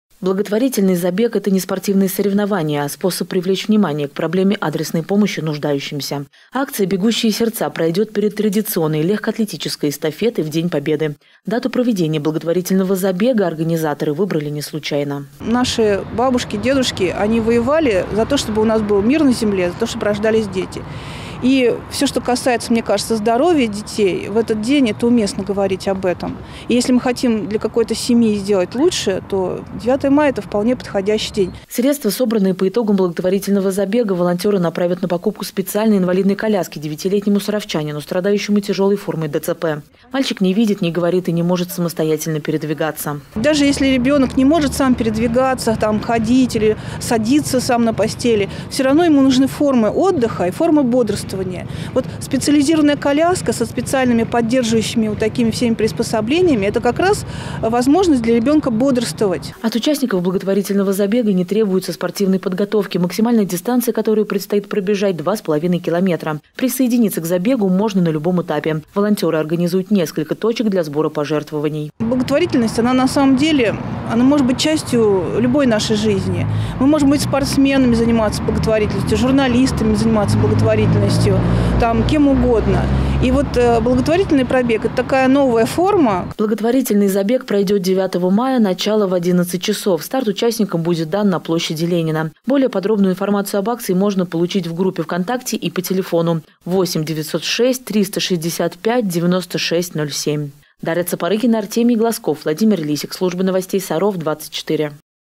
Бегущие сердца. Анонс
9 мая на площади Ленина пройдет благотворительный легкоатлетический забег «Бегущие сердца». Все собранные средства будут направлены на приобретение специальной инвалидной коляски для 9-летнего саровчанина.